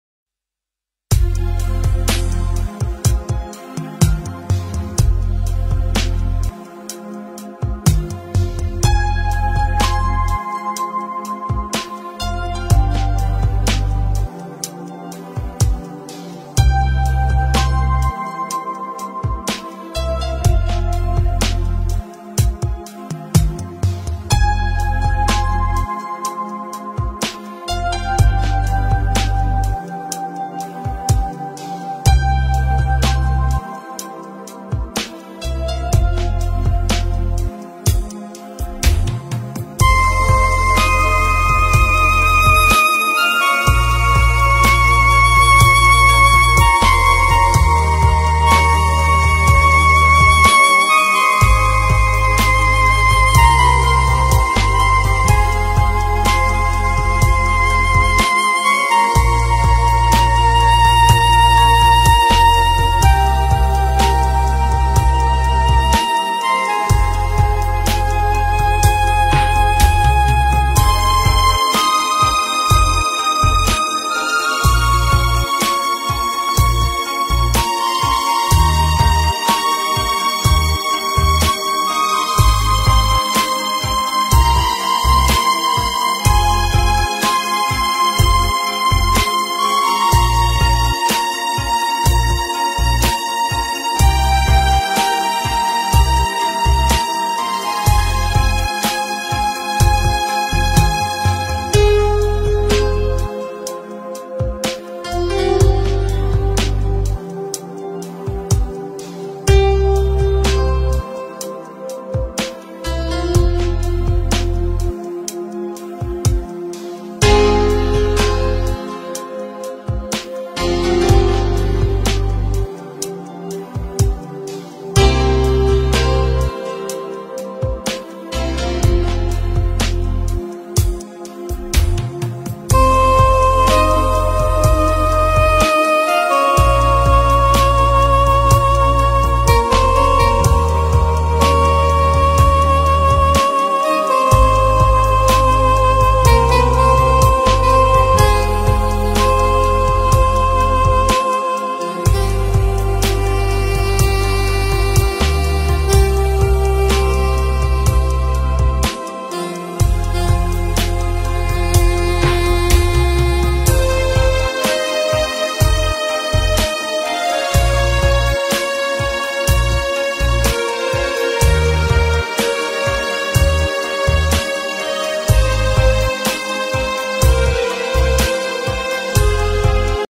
Rasht-Tajikistan
Имкониятҳои сайёҳии зимистонаи дараи Камароб

Бародарон бадустони худ тавсия диҳед канали моро.